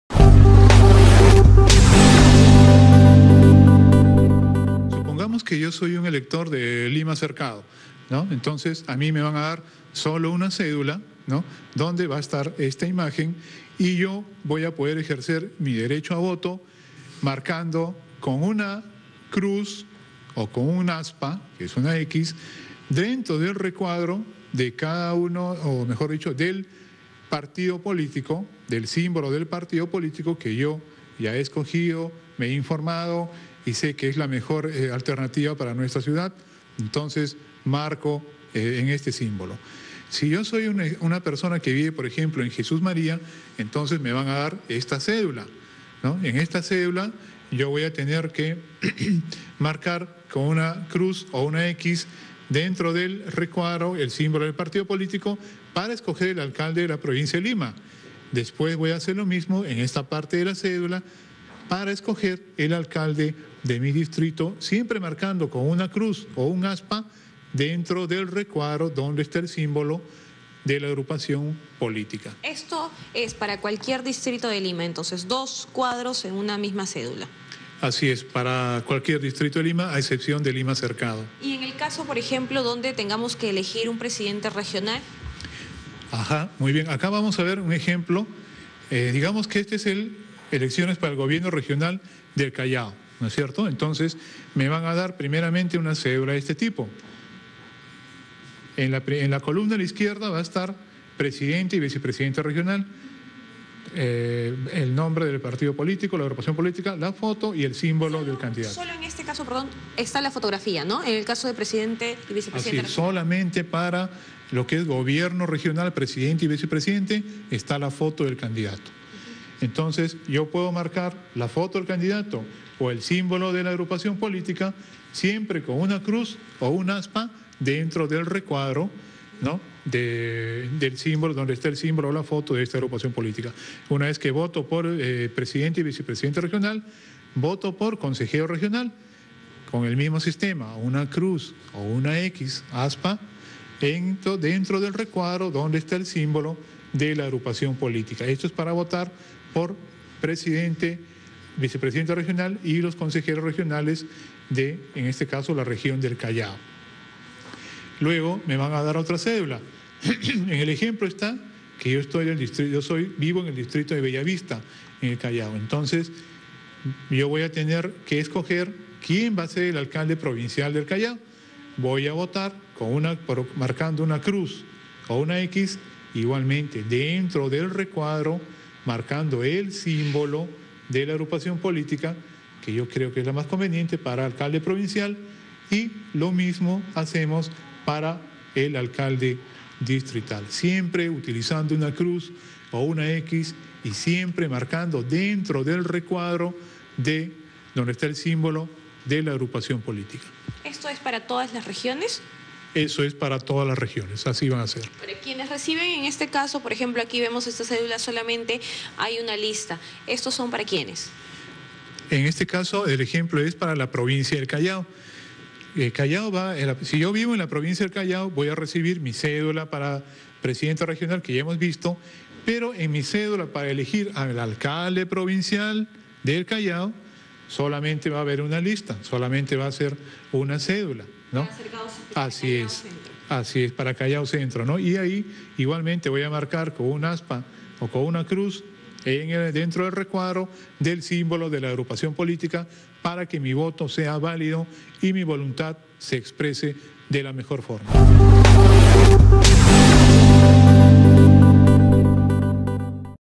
ONPE explica cómo se debe marcar en la cédula de votación
La Oficina Nacional de Procesos Electorales (ONPE) explicó cómo es que los ciudadanos deberán marcar en las cédulas de sufragio a la hora de emitir su voto este domingo 5 de octubre.